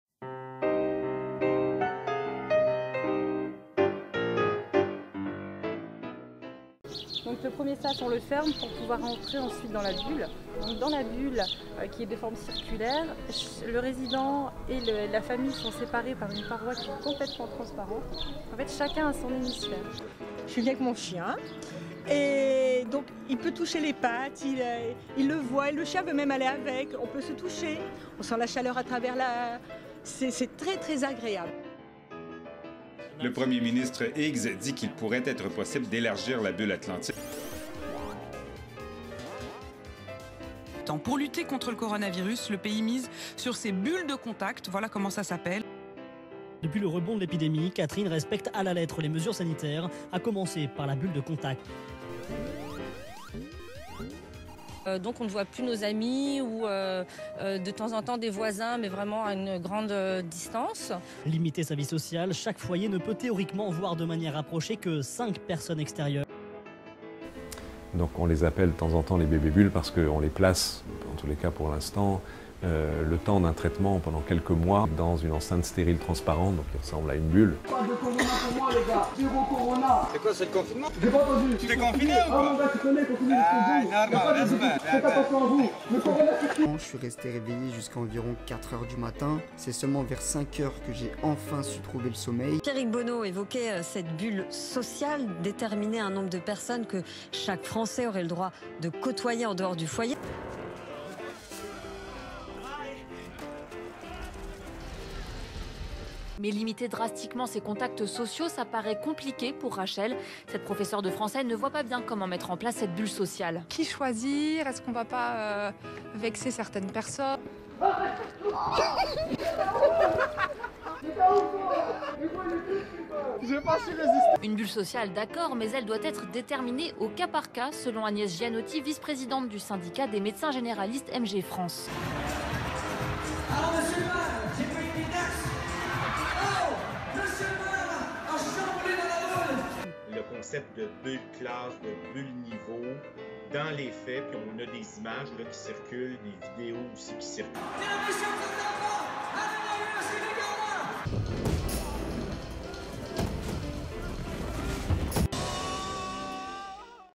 COVID-19 : la gouvernance des bulles
Un nouveau ''mème'' langagier, la bulle. Concept à décortiquer, comprendre et analyser.